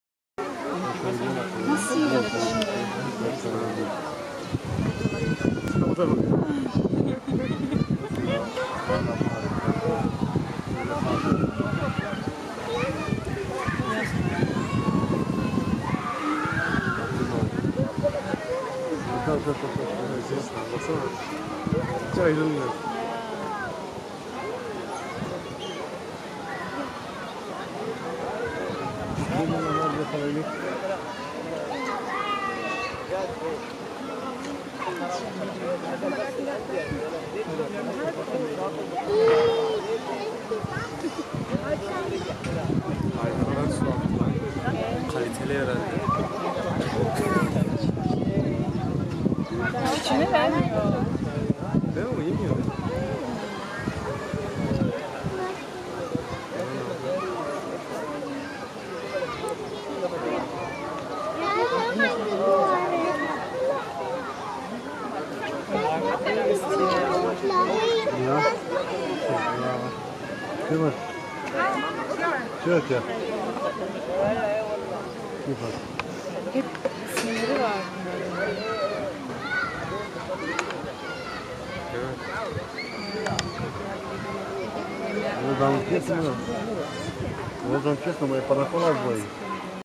Mother elephant breaking watermelon with mouth and one month old babyelephant breastfeeding
At first you can see how the little cute babyelephant walking under elephantmother and then how she is breastfeeding her babygirl with milk